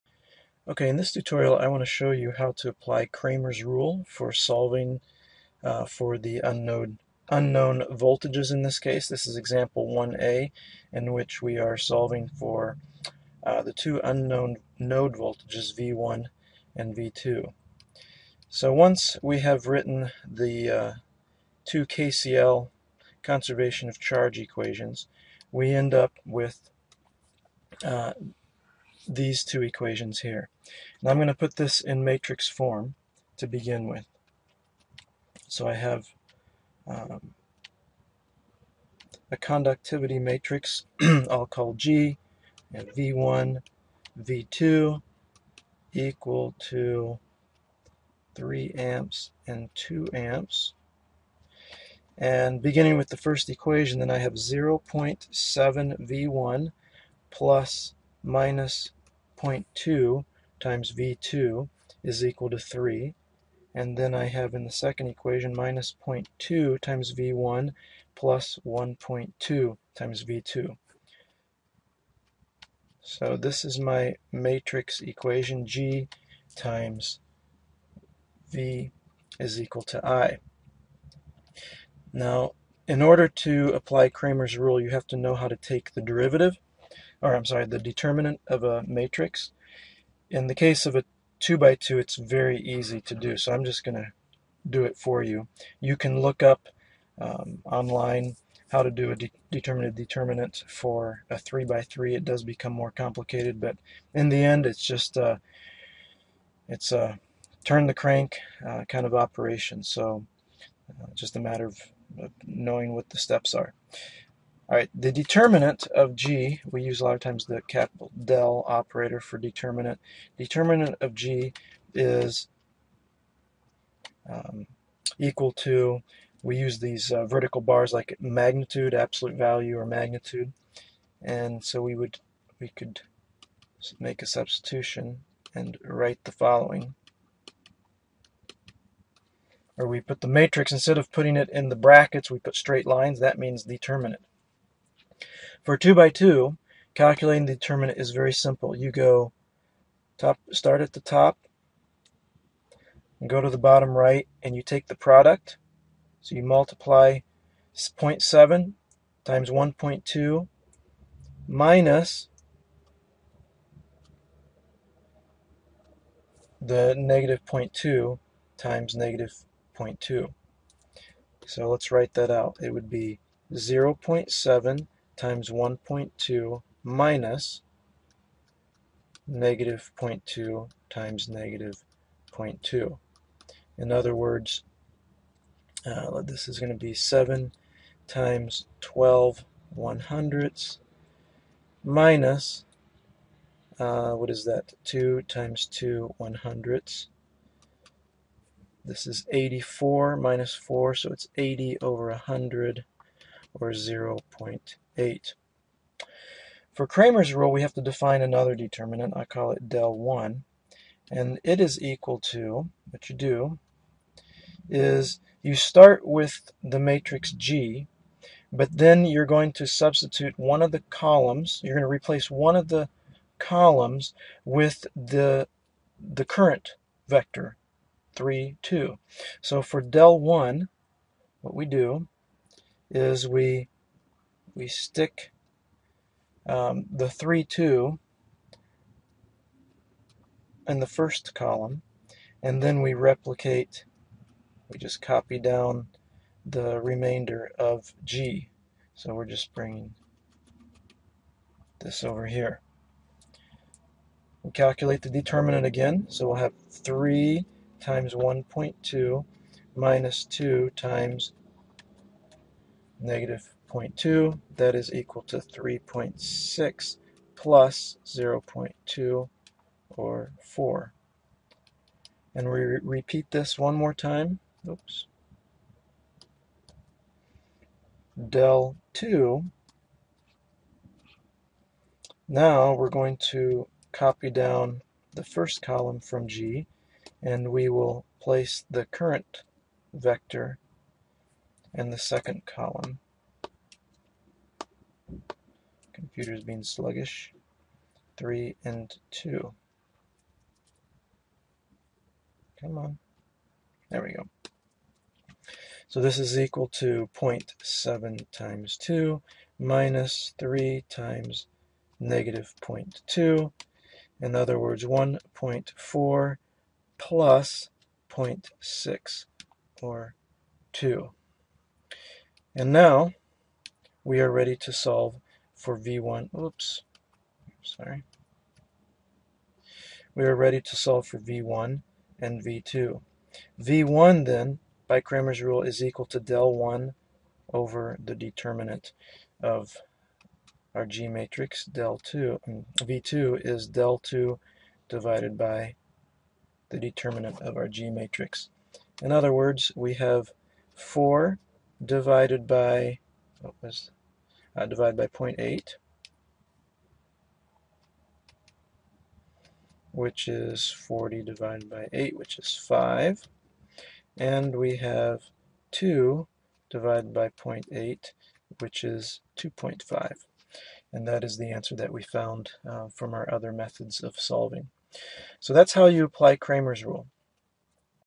Cramer's rule for 2x2 Circuit (LET05)
Solves a set of two nodal equations using Cramer's rule.